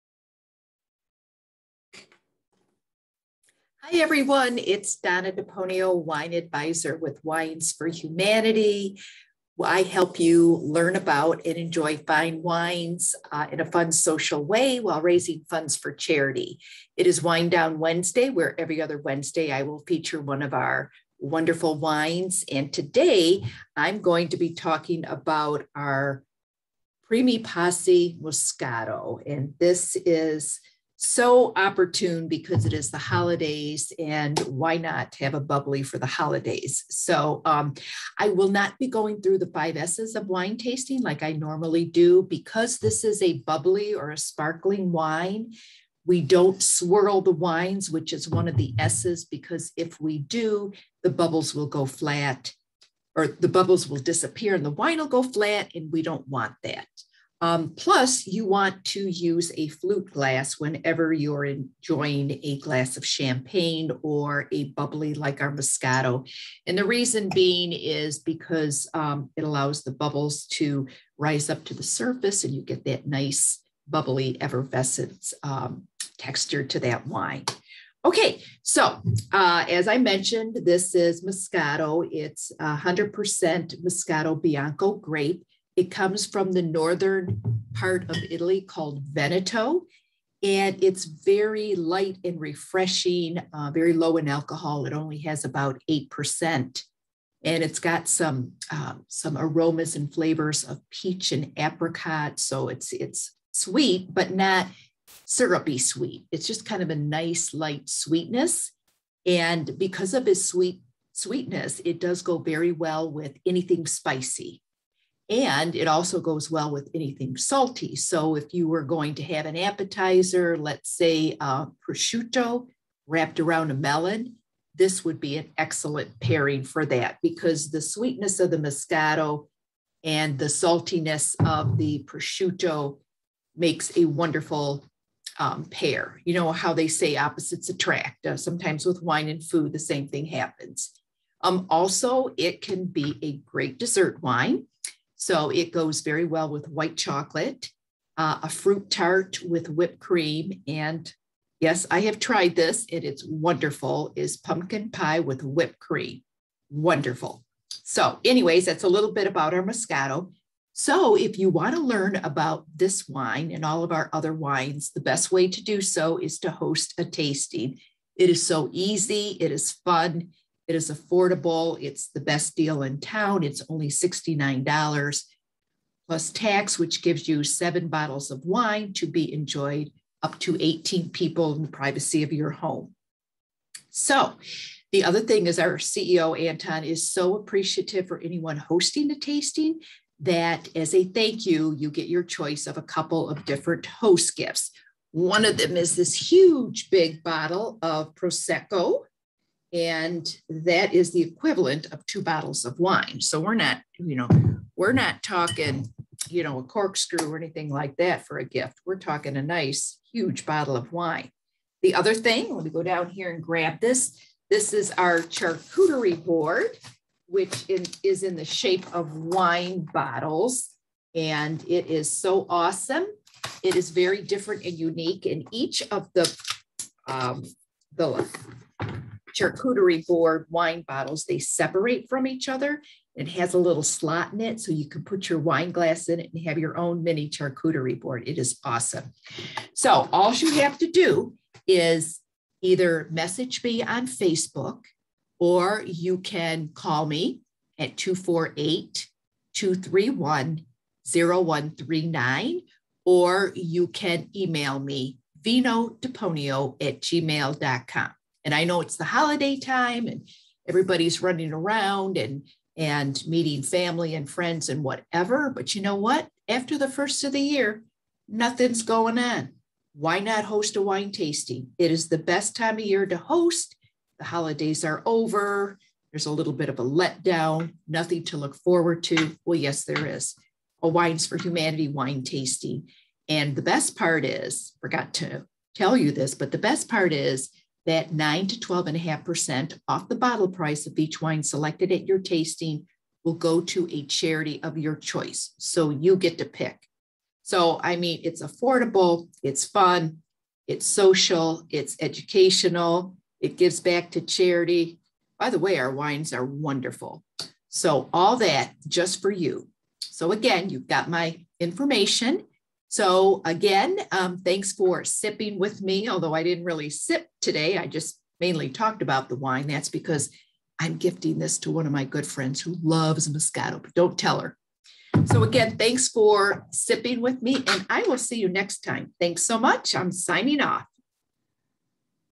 Learn about one of our bubbly wines – our Primi Passi Moscato from Italy
Our Primi Passi Moscato is great way to celebrate to holidays and can be used as an aperitif or as a dessert wine with pumpkin pie and whipped cream.